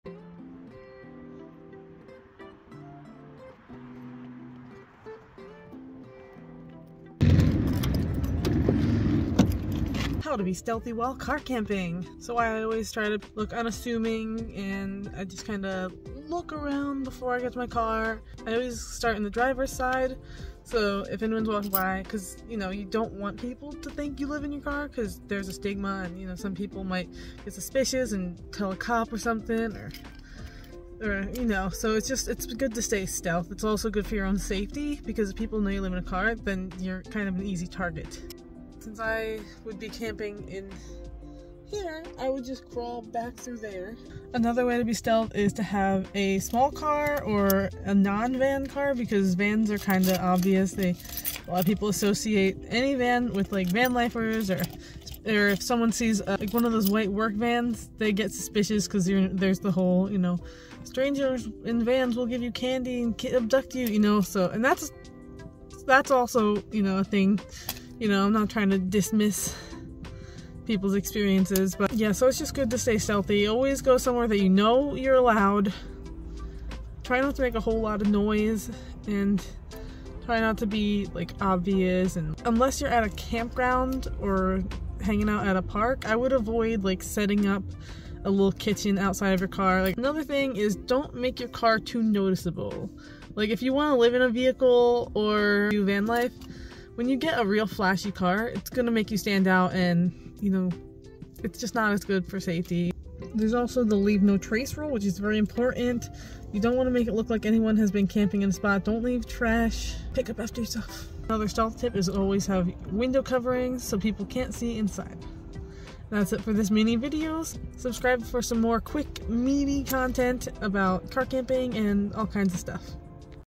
Stealth Camping Tips | Urban Car Camping
some quick tips on how to be stealthy while car camping! i forgot to mention: set up your bed somewhere separate from where you're actually sleeping.

Car camping stuff

Water dispenser
Side seat organizer
Power banks

Mermaidliv.com